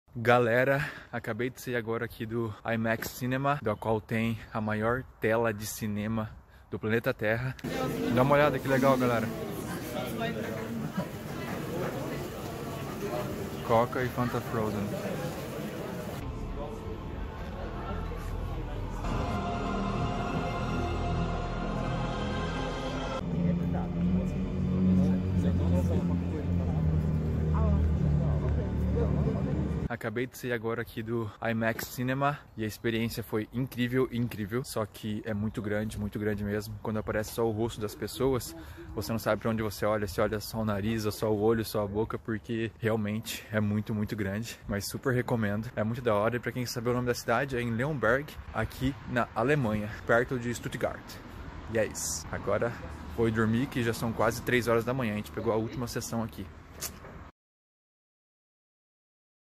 A maior tela de cinema do mundo
Com 21,03 metros de altura e 38,16 metros de largura, está posicionada no Traumpalast Multiplex, casa localizada na cidade de Leonberg, região sul da Alemanha. Para fins de comparação, ela é mais comprida do que um avião Boeing 737.